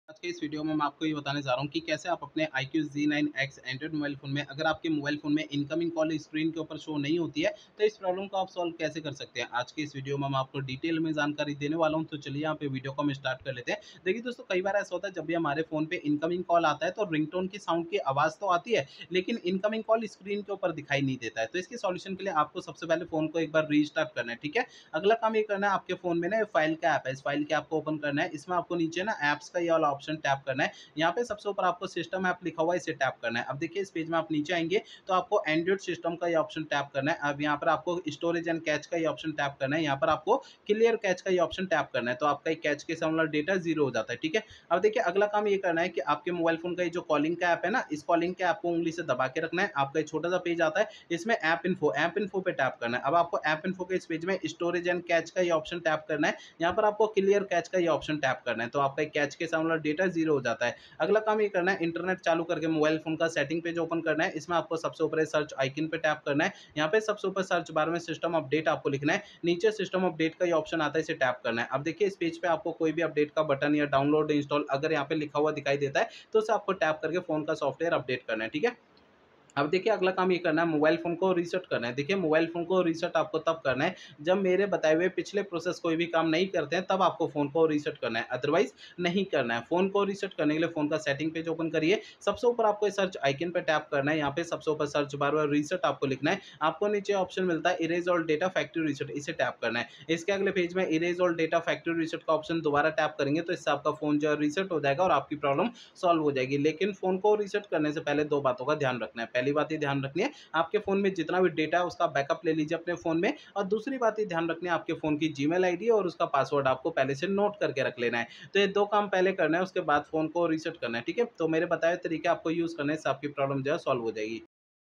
incoming call ringing but not showing IQOO Z9x, how to fix incoming call not showing on screen
Best powerful CPU under 10k

60-Inch Tripod with Bag

7 Socket 6A Extension Board